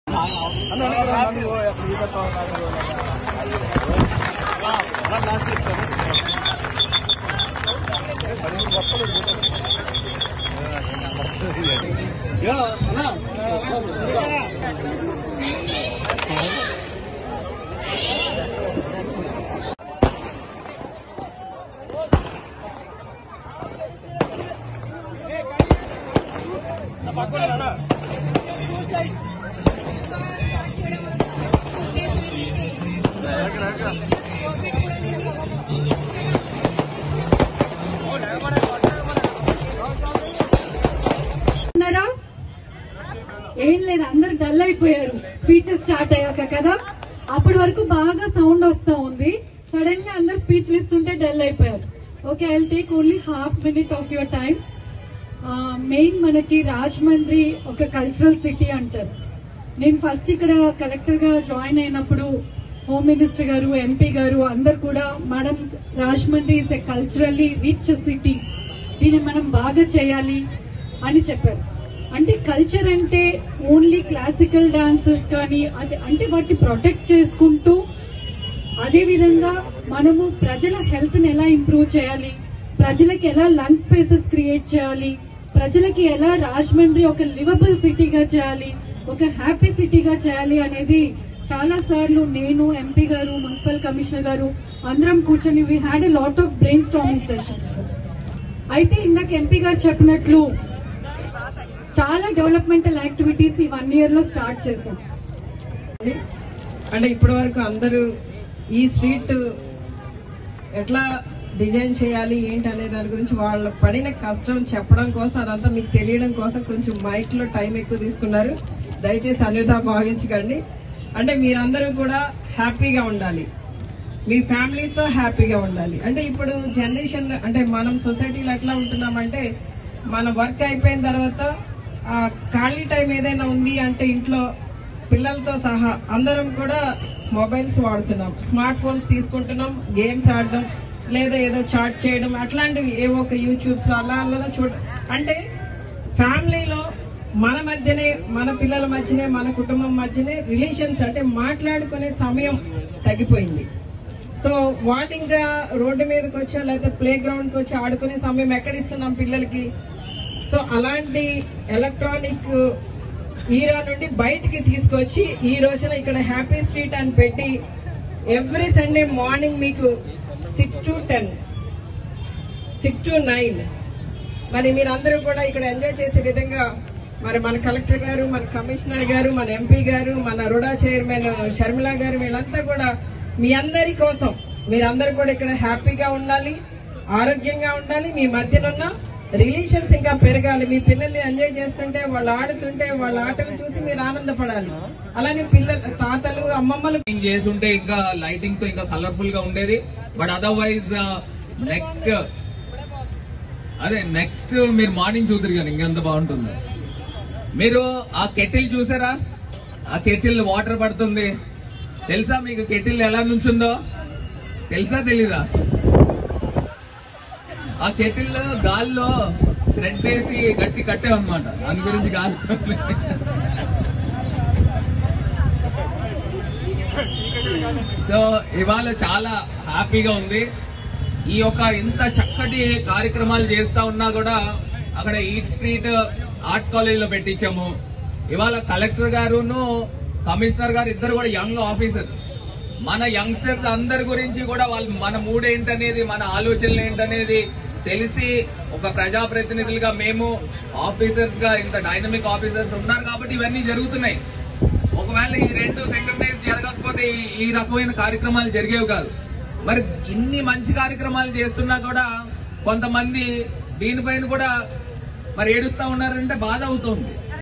హ్యాపీ స్ట్రీట్ ప్రారంభోత్సవం కార్యక్రమంలో రాష్ట్ర హోంశాఖ మంత్రి | MP Bharat About Taneti Vanitha
Watch : హ్యాపీ స్ట్రీట్ ప్రారంభోత్సవం కార్యక్రమంలో రాష్ట్ర హోంశాఖ మంత్రి | MP Bharat About Taneti Vanitha  @Hittvtelugunews